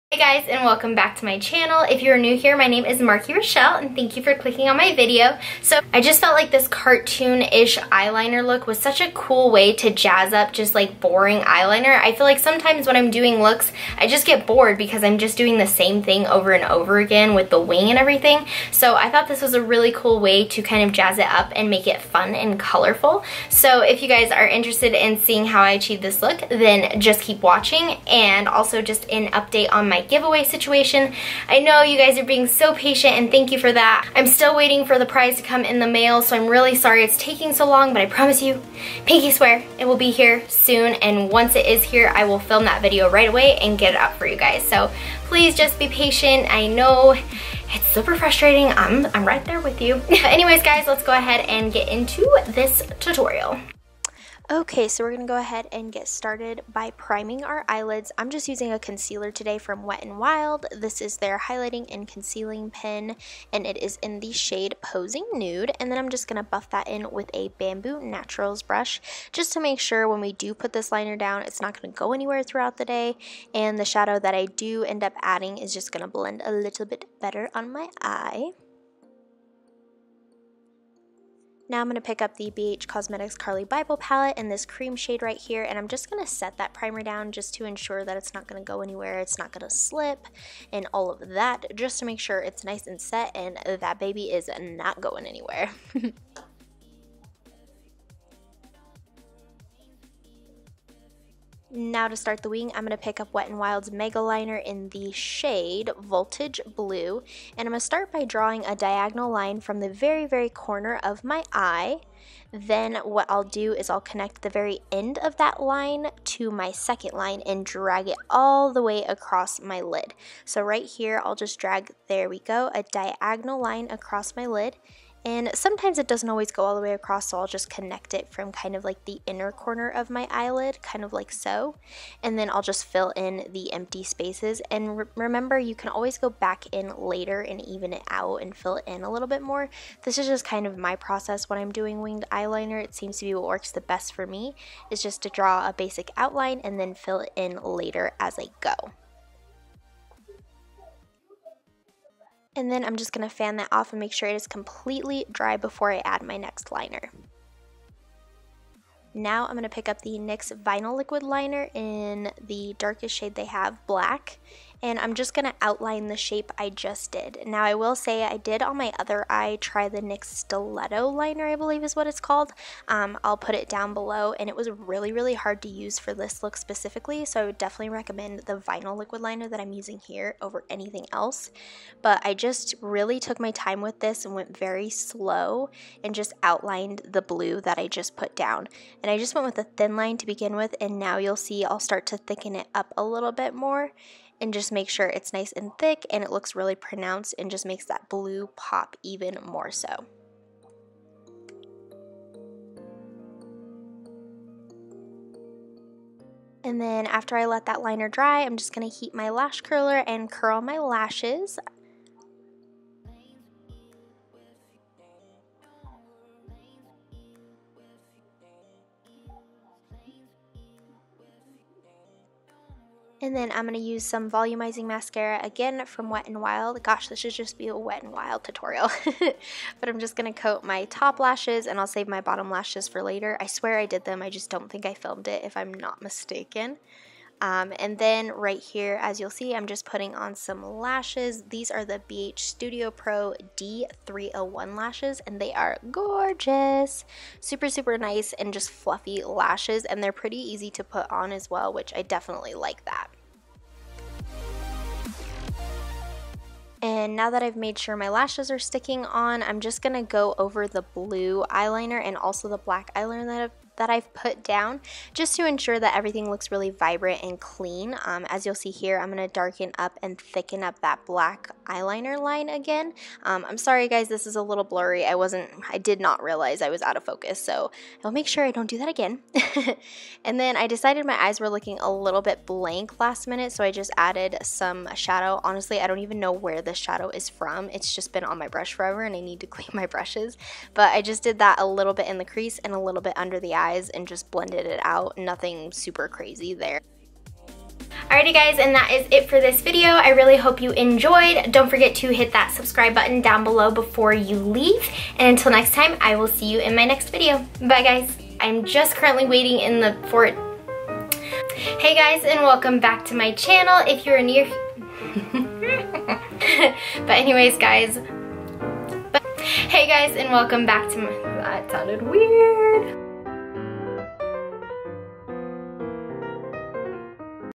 Cartoon Eyeliner Tutorial | Marki Rochelle
Hello friends! I thought I would give this new cartoon eyeliner trend a go and I thought it turned out super cool! I hope you love it!

Products Used:

Primer: Wet N Wild "Highlighting & Concealing" Pen (Posing Nude)

Shadows: BH Cosmetics "Carli Bybel" Palette

Liners: NYX "Vinyl" Liquid Liner, Wet N Wild "Megaliner" Liquid Liner (Voltage Blue)

Mascara: Wet N Wild "Mega-Volume" Mascara (Black)

Lashes: BH "Studio Pro" Lashes (D-301)

Brushes

Morphe Brushes (most of mine don't have numbers), Bamboo Naturals Smudge Brush

Music